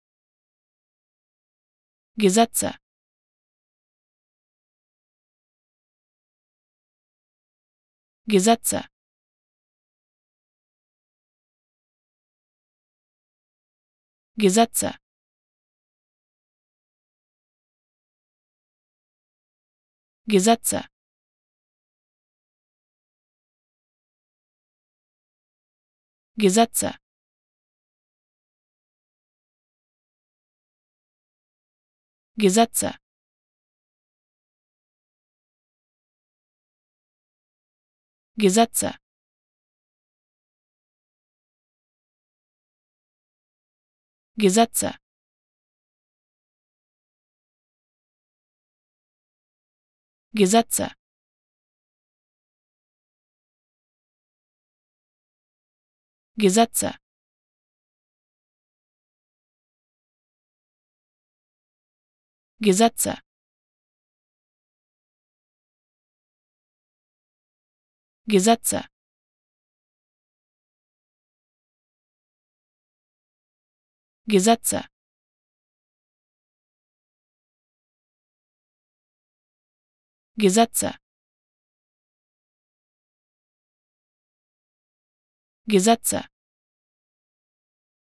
How to Pronounce ''Gesetze'' (Laws) Correctly in German
Learn how to say ''Gesetze'' in German with this short pronunciation tutorial.